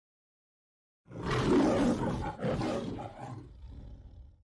if the 2021 MGM closing logo was animated
oh and I also animated the shiny filmstrips, it may look a bit odd but it was a pain though to get it done correctly.